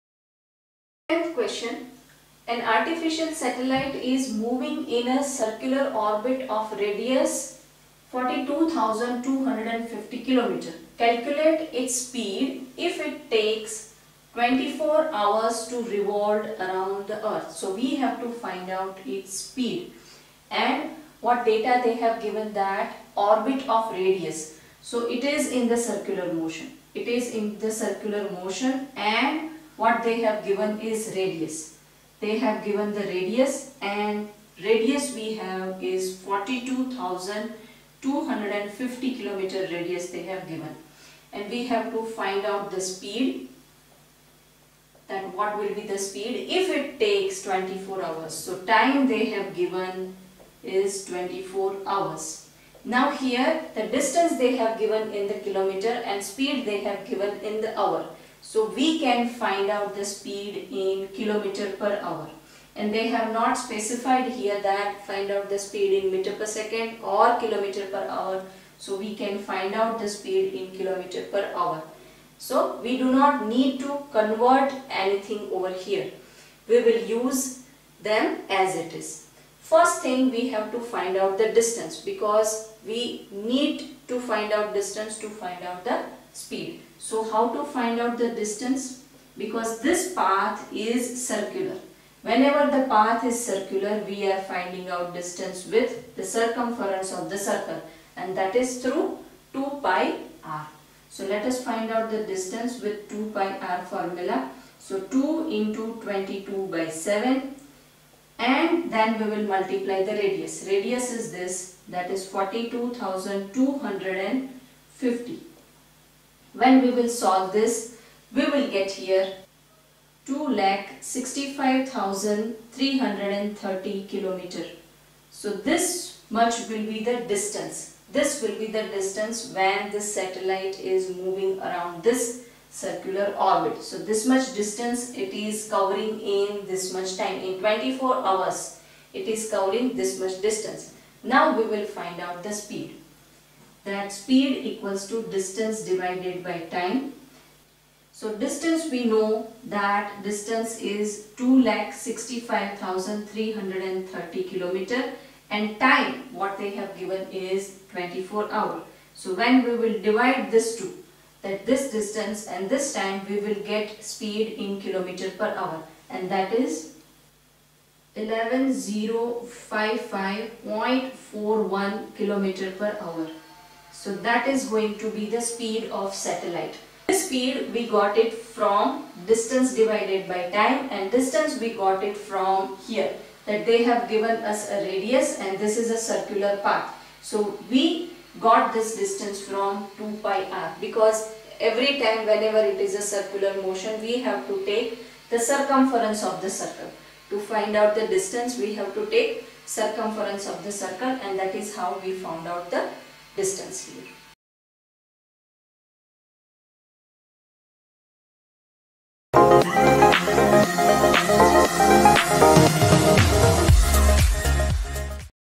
CBSE Std-9 Science Ch-8 MOTION Exe Que no.10 || Exercise Solution ||NCERT Science Class 9
10. An artificial satellite is moving in a circular orbit of radius
42250 km. Calculate its speed if it takes 24 hours to revolve
around the earth.